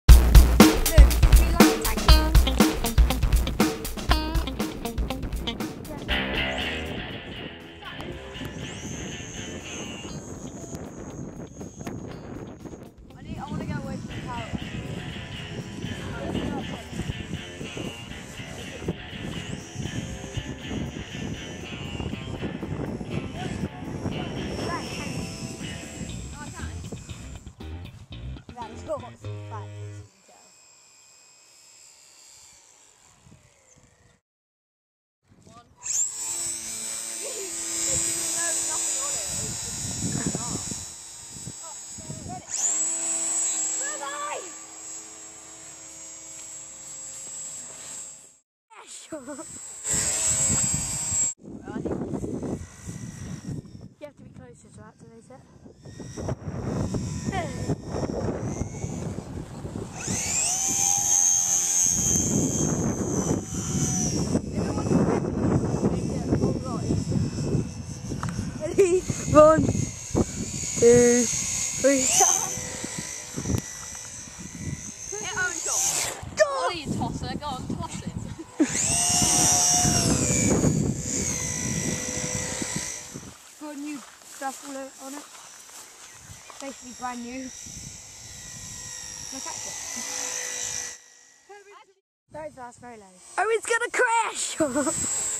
Best of the Super Cub lp
Best shots of the old super cub before i lost it in a forest! Just a bit of a montage, enjoy.